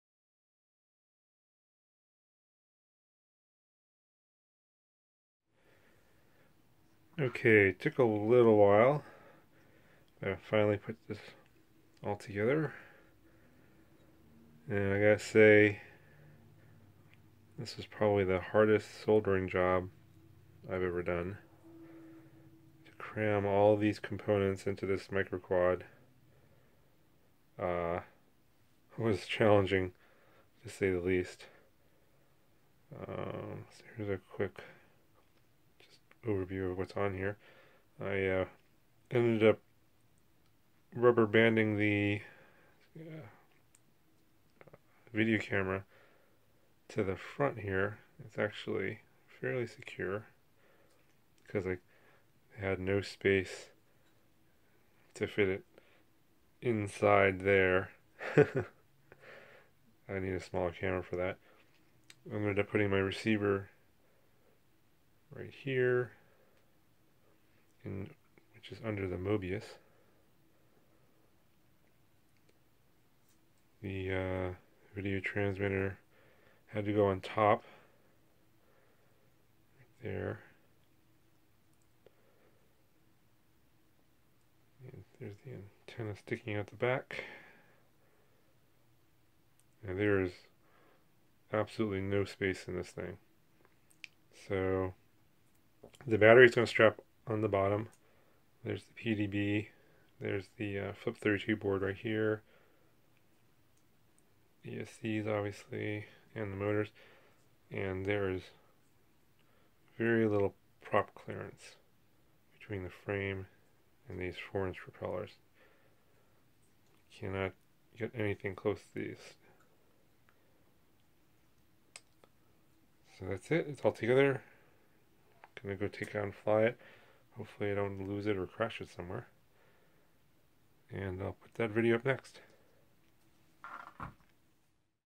Diatone Grasshopper 160 Build Overview
A brief overview of how I built the Diatone Grasshopper 160 microquad.  Cramming all these components into this tiny quad was quite challenging!

Parts List:
Diatone Grasshopper 160 G160 Carbon Fiber Quadcopter Frame Kit w/ BEC Power Distribution Board

Eachine 1306 BG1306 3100KV Motor CW/CCW For Diatone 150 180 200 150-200 Glass Frame Kit

KINGKONG BLHeli 12A Brushless High Speed ESC With OneShot125 For FPV Multicopter

2 Pairs DAL 4045 4x4.5 Inch PC Fiberglass Multicolor Propeller CW/CCW For Multicopter

Eachine 700TVL 2.6mm Lens 1/3 CMOS 110 Degree FPV Mini Camera

RP-SMA Male to RP-SMA Female Adapter Right Angle RF Connector
Fatshark Dominator V3 Goggles

What I use to film my videos